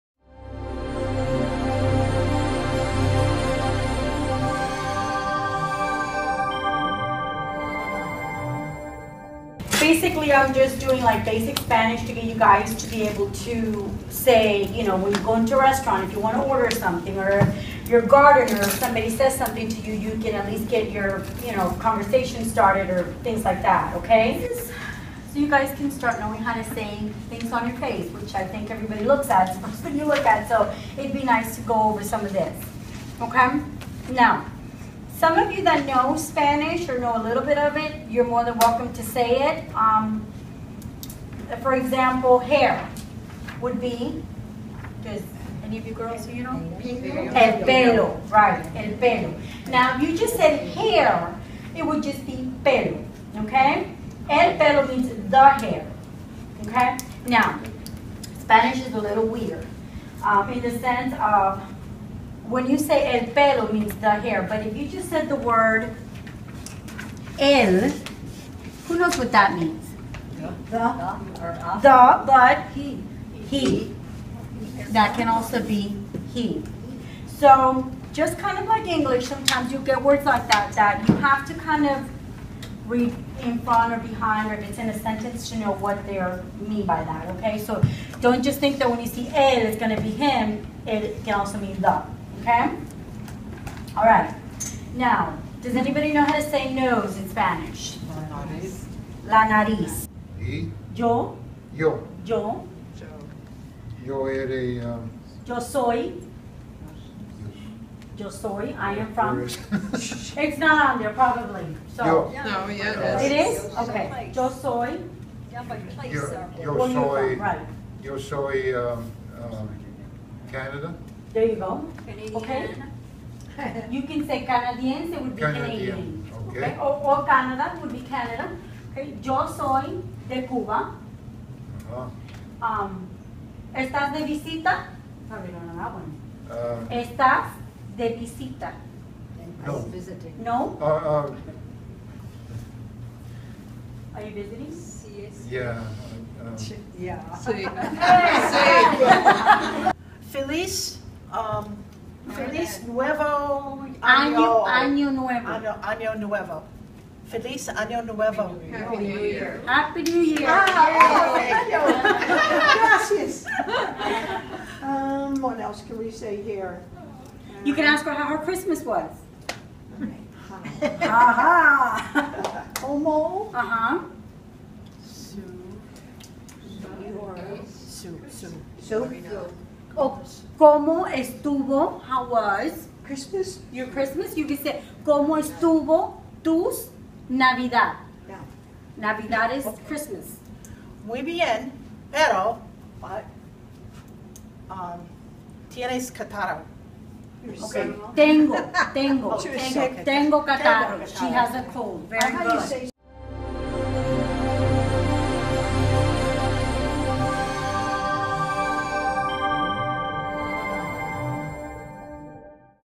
Basic Spanish Class at South Manatee Branch Library
Bradenton resident Maribel Acosta teaches Basic Spanish on a recent afternoon to locals at South Manatee Branch Library in Bradenton.